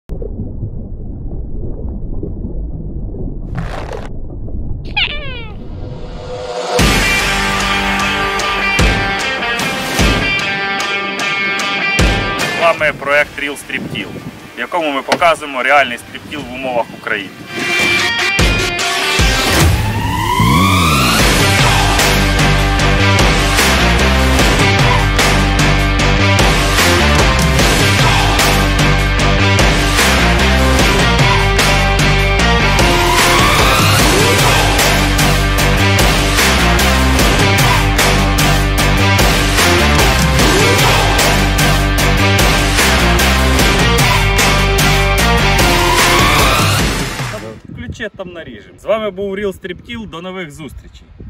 Real Strip Till Promo / Реальний стріп тілл АНОНС
Очікуйте виходу першої серії проекту "Реальний стріптіл" де ми покажемо всі труднощі впровадження технології стріптіл в українських реаліях.